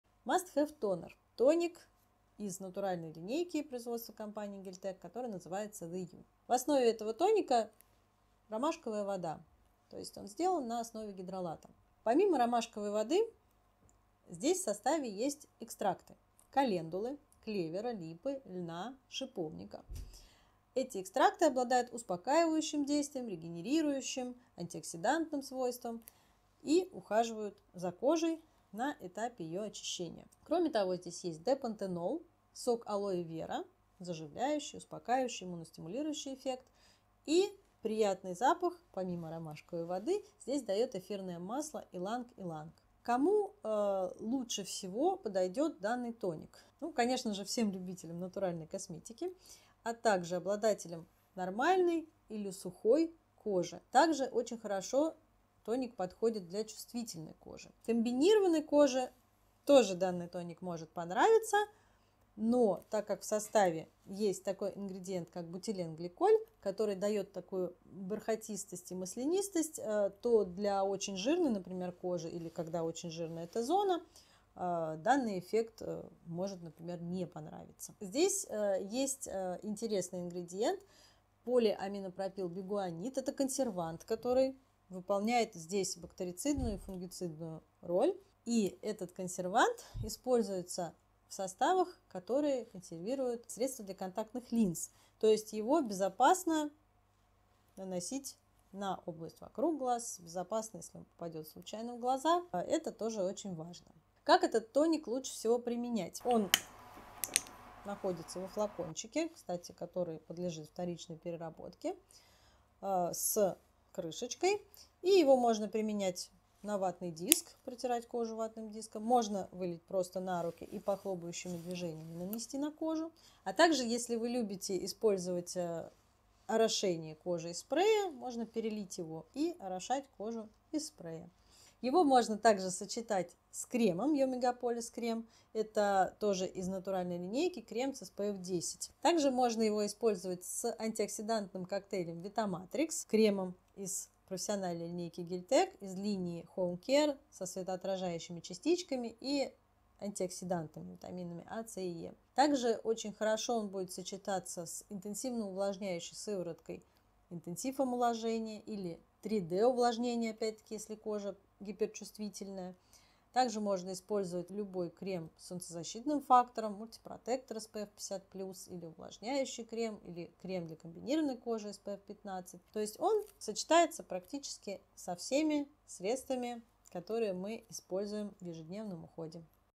The U Тоник успокаивающий Must have toner
Натуральный успокаивающий тоник для ухода за любым типом кожи. Содержит в составе комплекс экстрактов лекарственных трав: календулы, ромашки, липы, льна,липы и клевера, который увлажняет и тонизирует кожу, защищает от негативных факторов окружающей среды, делает ее более гладкой и сияющей. Пантенол восстанавливает и заживляет, алоэ снимает раздражения и увлажняет. Тоник имеет легкую текстуру и мгновенно впитывается, не оставляя неприятного ощущения пленки.

За счёт эффективного успокаивающего состава тоник можно применять ежедневно, особенно в период использования кислот, ретинола и других раздражающих компонентов. Летом Must have toner особенно актуален: он успокоит кожу после воздействия солнца, снимет раздражение.

Состав: ромашковая вода, пропиленгликоль, бутиленгликоль, полиглицерил-4 капрат, экстракт календулы лекарственной, экстракт клевера лугового, экстракт липы сердцевидной, экстракт льна обыкновенного, экстракт шиповника, глицерин, пантенол, сок алоэ вера, эфирное масло иланг иланга, полиаминопропилбигуанид

НОВИНКИ! __________________________________________